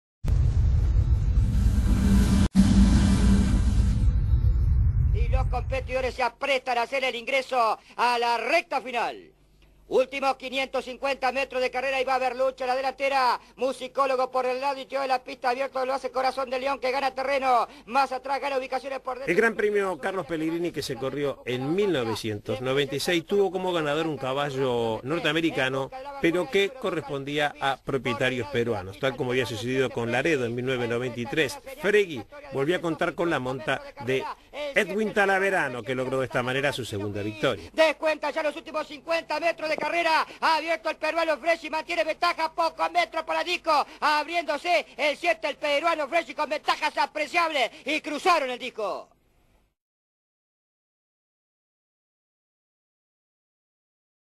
FREGY'S - GRAN PREMIO INTERNACIONAL CARLOS PELLEGRINI 1996
Edwin talaverano y otra maestra conducción en el hipódromo de san isidro